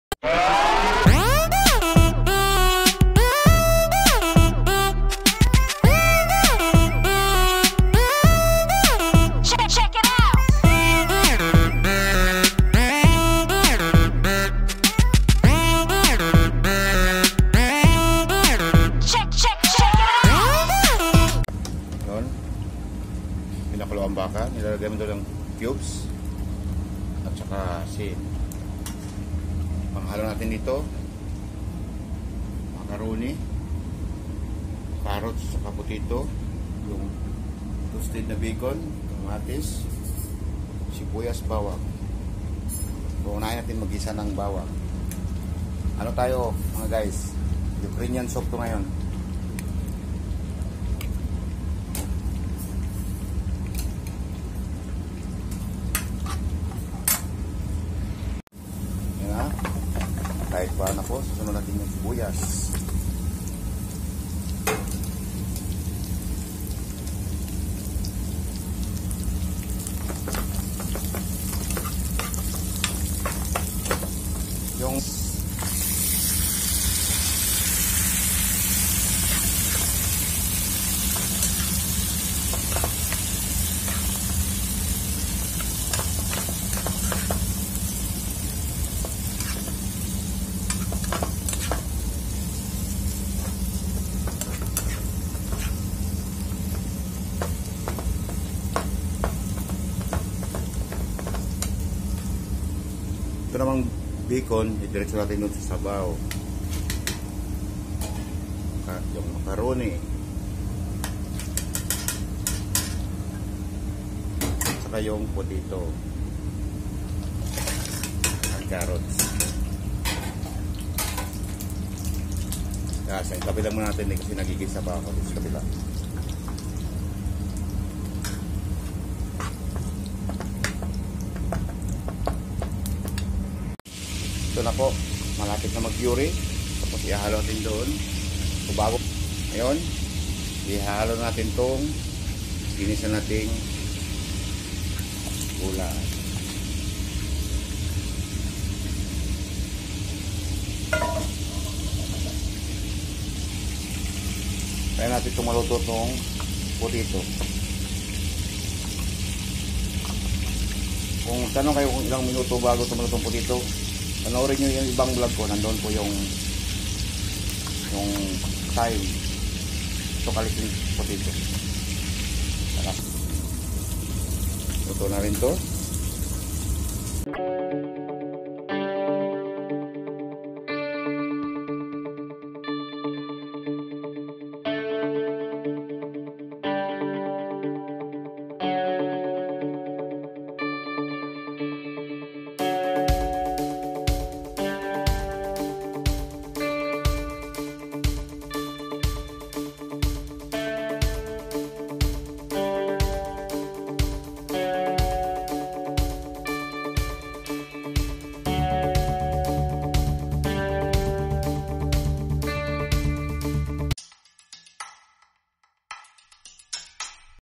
BACON AND BEEF SOUP (UKRAINIAN STYLE)
bacon and beef soup is simlpe soup  in Ukraine  this is one most popular  soup in Odessa